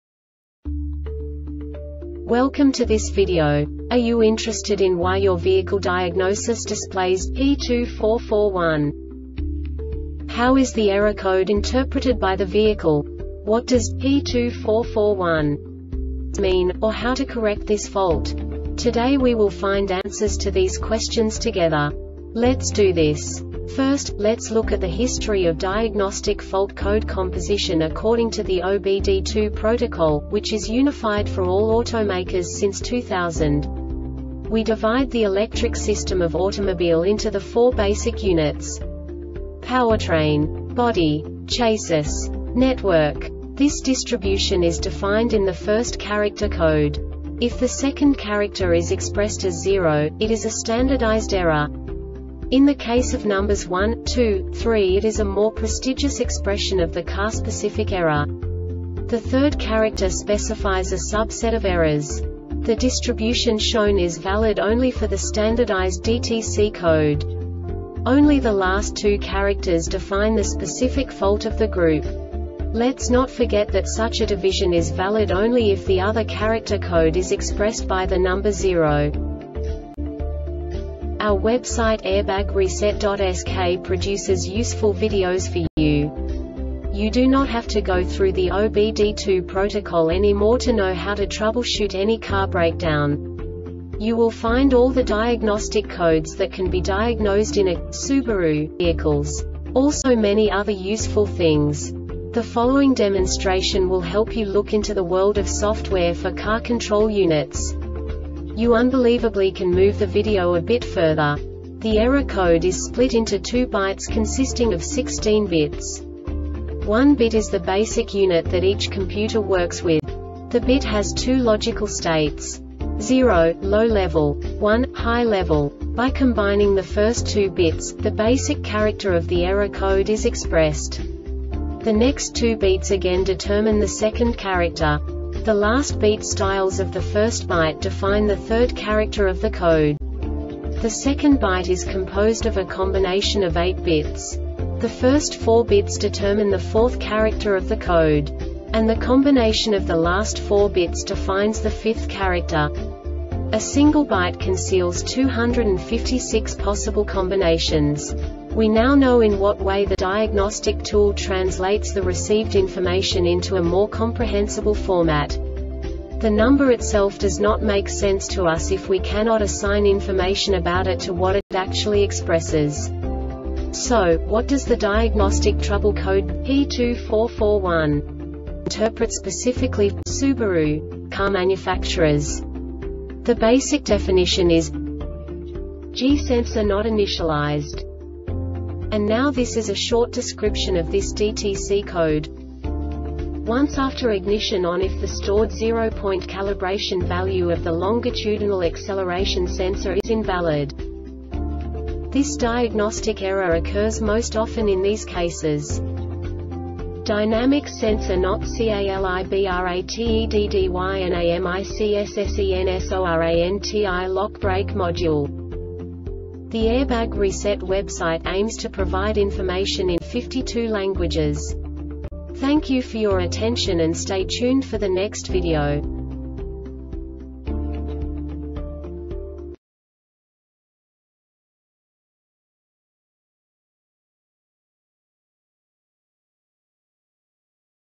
DTC Nissan P2441 Short Explanation
Contents:
0:21 Basic DTC analysis according to OBD2 protocol standard.
1:48 Insight into programming
2:58 A diagnostically understandable description of the Diagnostic Trouble Code
3:05 Basic error definition

Make
Subaru
Code
P2441
Definition
G SENSOR NOT INITIALIZED
Description
Once after ignition on If the stored zero point calibration value of the longitudinal acceleration sensor is invalid
Cause
DYNAMICS SENSOR NOT CALIBRATEDDYNAMICS SENSORANTI-LOCK BRAKE MODULE